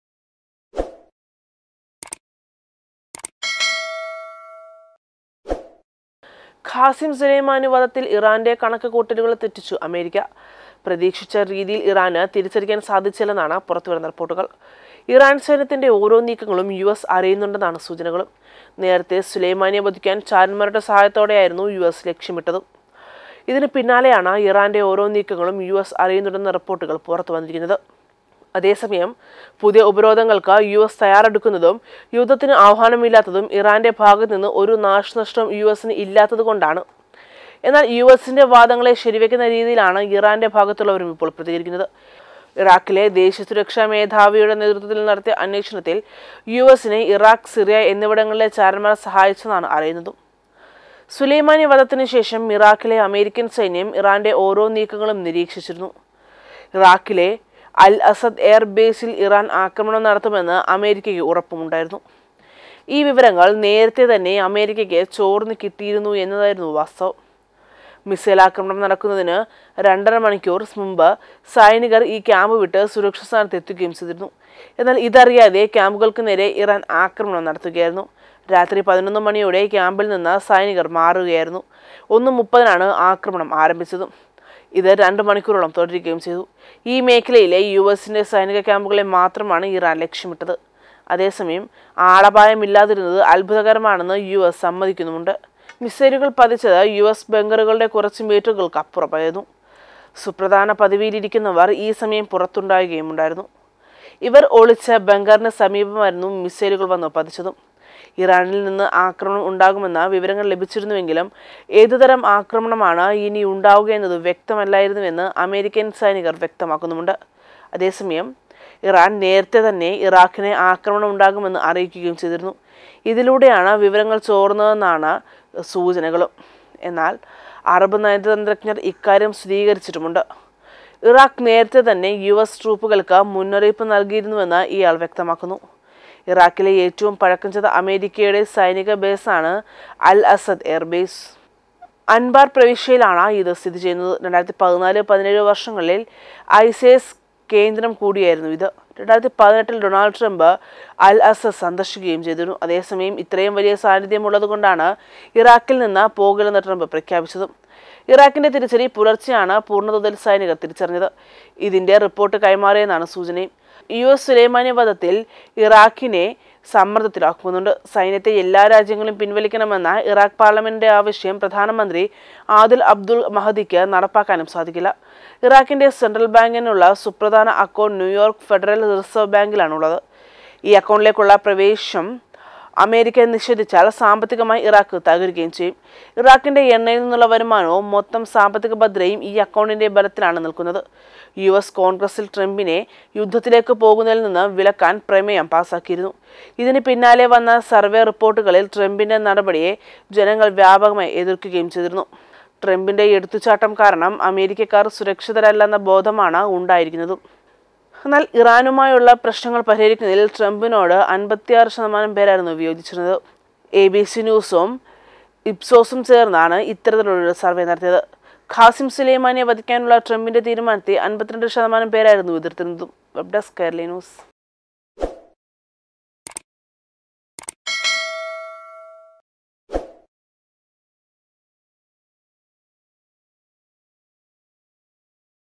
ഇറാന്റെ രഹസ്യം യുഎസ് ചോര്‍ത്തുന്നു മിന്നലാക്രമണത്തിന് മുമ്പേ സൈനികരെ മാറ്റി | Iran-US War And Trump
ഖാസിം സുലൈമാനി വധത്തില്‍ ഇറാന്റെ കണക്കുകൂട്ടലുകള്‍ തെറ്റിച്ച് അമേരിക്ക. പ്രതീക്ഷിച്ച രീതിയില്‍ ഇറാന് തിരിച്ചടിക്കാന്‍ സാധിച്ചില്ലെന്നാണ് റിപ്പോര്‍ട്ടുകള്‍. ഇറാന്‍ സൈന്യത്തിന്റെ ഓരോ നീക്കങ്ങളും യുഎസ് അറിയുന്നുണ്ടെന്നാണ് സൂചന.